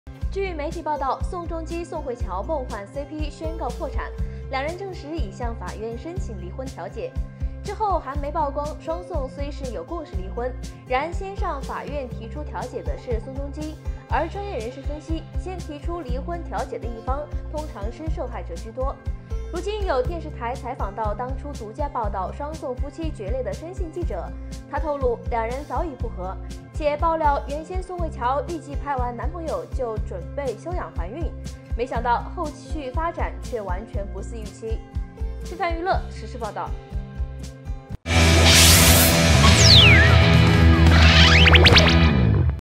【上個月】宋慧喬正在備孕中？韓媒公開雙宋夫婦離婚內情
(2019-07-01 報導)

宋慧喬正在備孕中？韓媒公開雙宋夫婦離婚內情